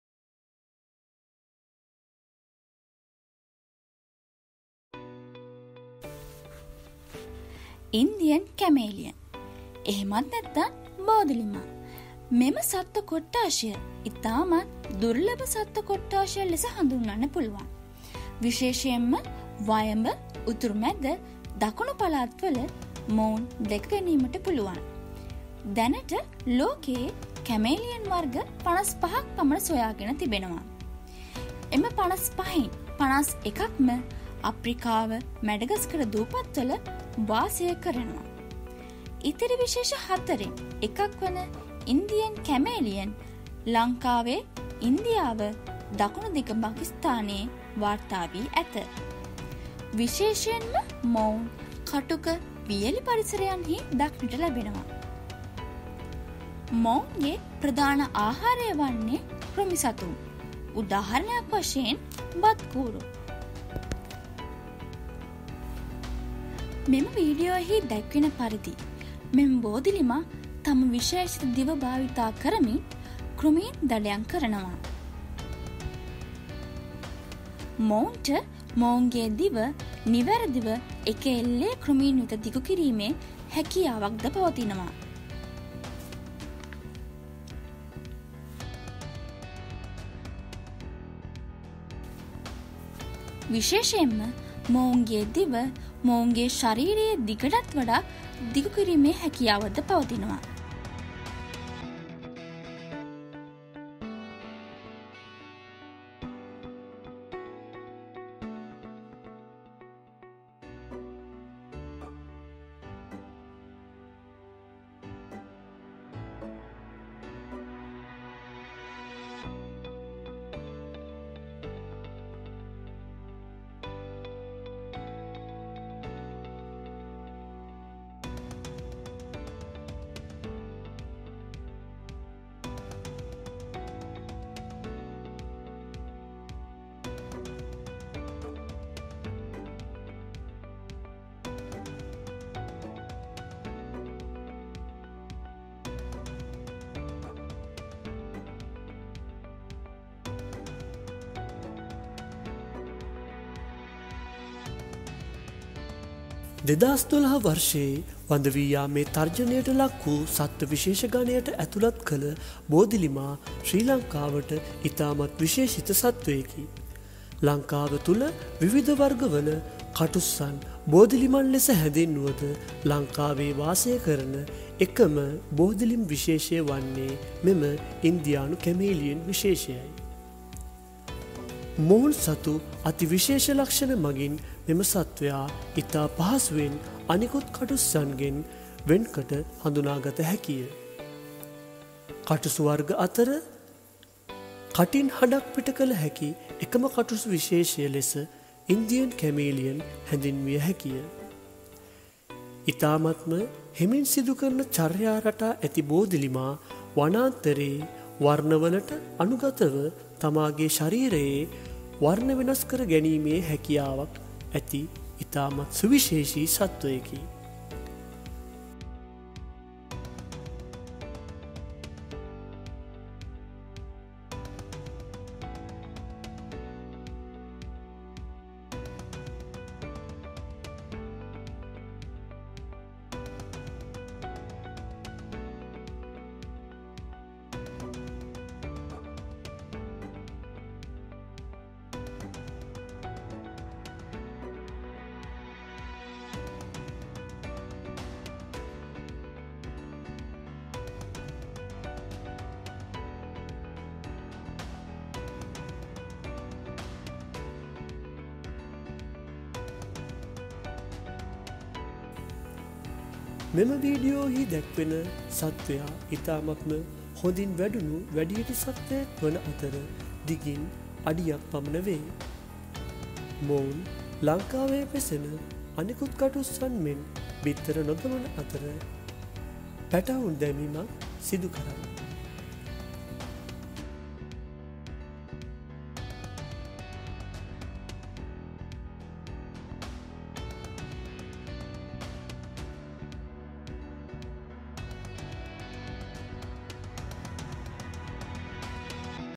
Indian Chameleon (Bodilima - බෝදිලිමා)
The Indian chameleon (Chamaeleo zeylanicus) is a species of chameleon found in Sri Lanka, India, and other parts of South Asia. Like other chameleons, this species has a long tongue, feet that are shaped into bifid claspers, a prehensile tail, independent eye movement, and the ability to change skin colour. They move slowly with a bobbing or swaying movement and are usually arboreal. Strangely, they do not choose the background colour and may not even be able to perceive colour differences. They are usually in shades of green or brown or with bands. They can change colour rapidly and the primary purpose of colour change is for communication with other chameleons and for controlling body temperature by changing to dark colours to absorb heat.